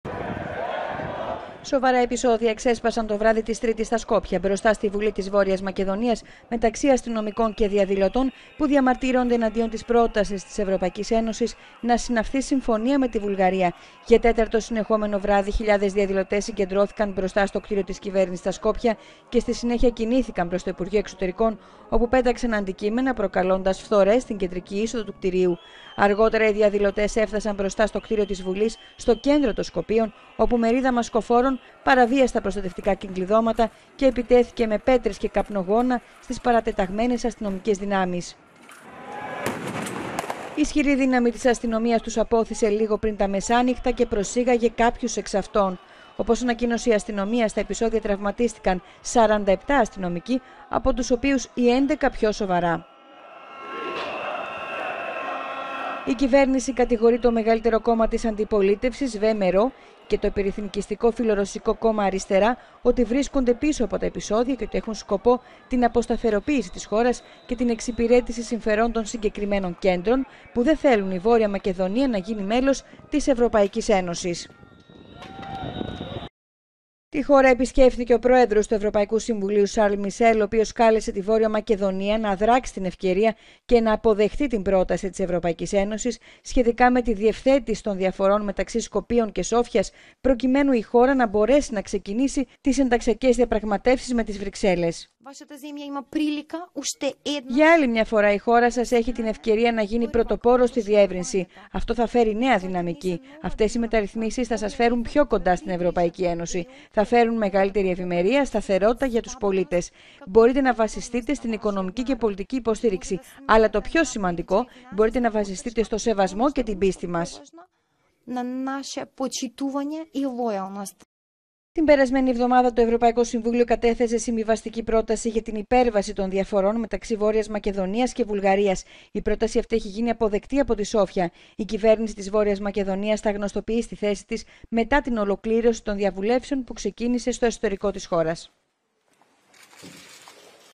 Σοβαρά επεισόδια στα Σκόπια σε διαδήλωση κατά της συμφωνίας με την Βουλγαρία
Τραυματίστηκαν 47 αστυνομικοί, από τους οποίους οι 11 πιο σοβαρά.